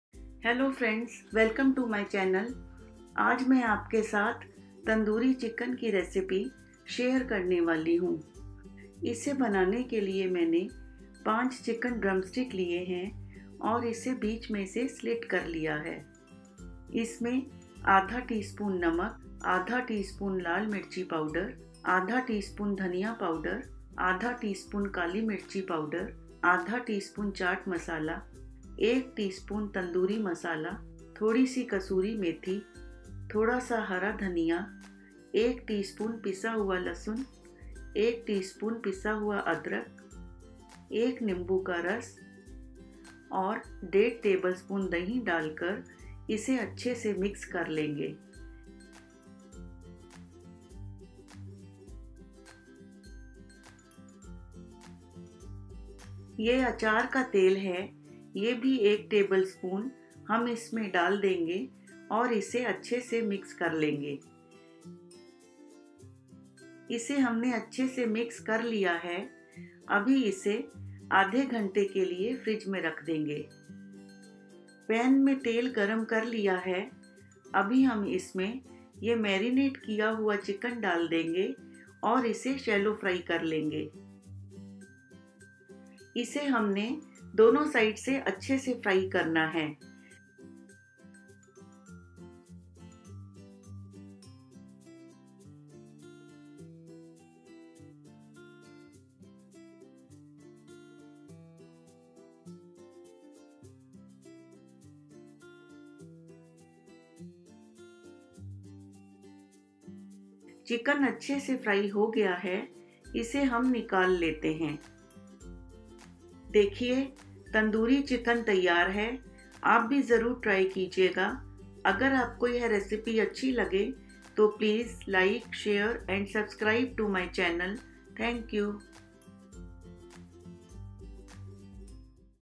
Easy & Quick Tandoori Chicken Recipe | Spicy Chicken Tangdi Kebab | Chicken Drumstick | AnitaDaDhaba
Ingredients:
5 chicken drumsticks
1/2 tsp salt
1/2 tsp red chilli powder
1/2 tsp coriander powder
1/2 tsp black pepper powder
1/2 tsp chaat masala
1 tsp tandoori masala
Kasuri methi
Coriander
1 tsp crushed garlic
1 tsp crushed ginger
1 lemon juice
1.5 tbsp curd
1 tbsp achaar/pickle oil